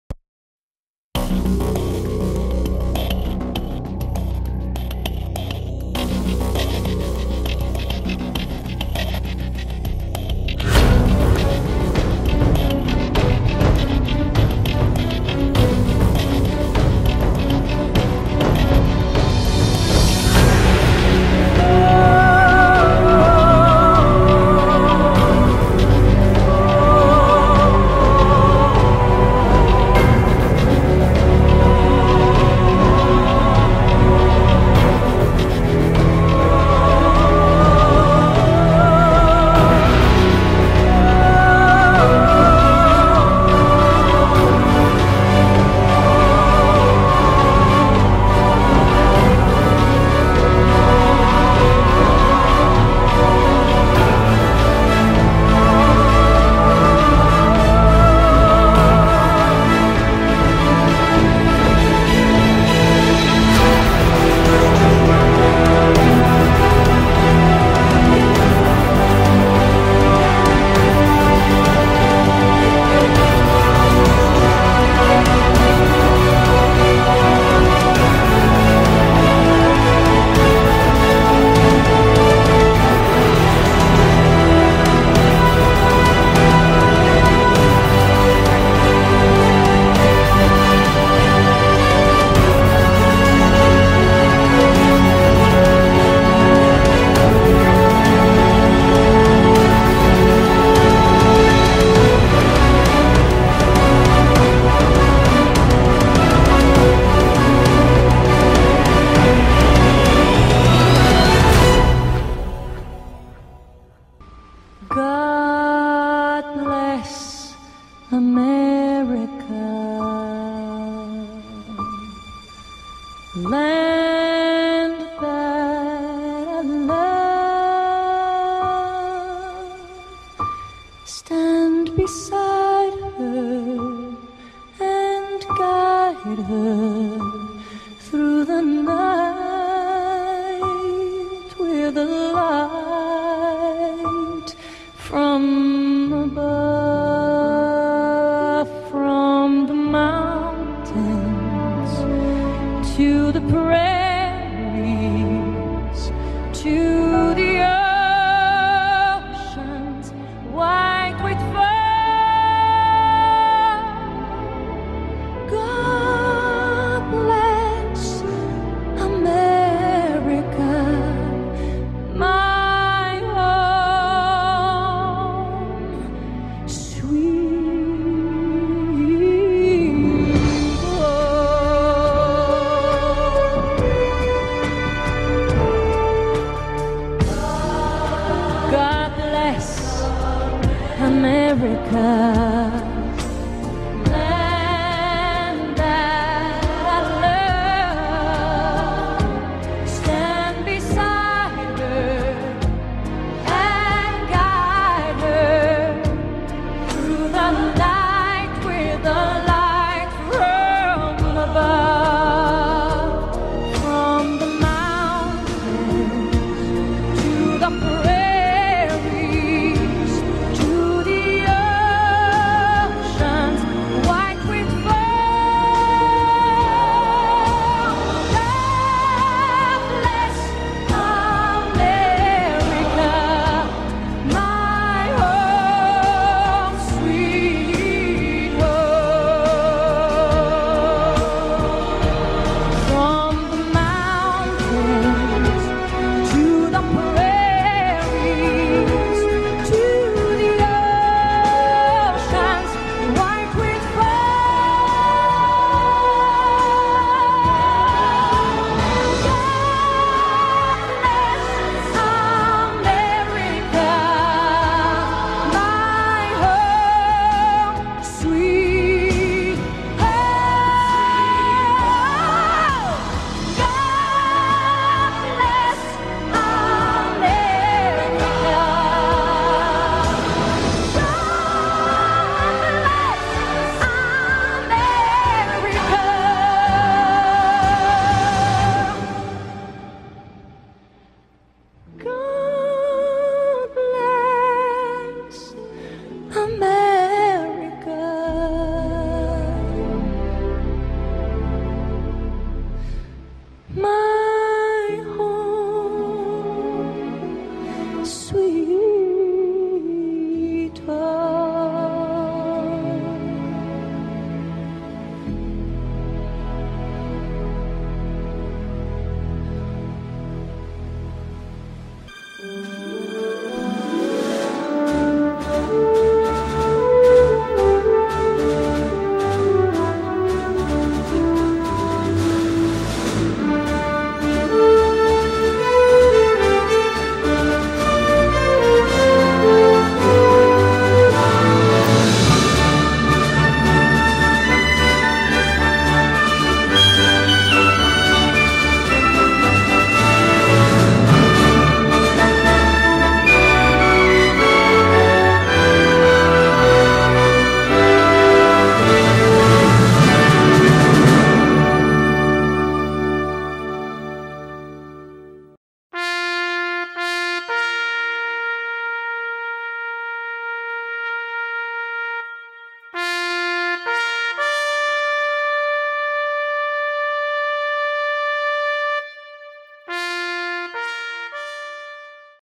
Veterans Day Tribute 2012 PFCC
A tribute to the veterans of our congregation.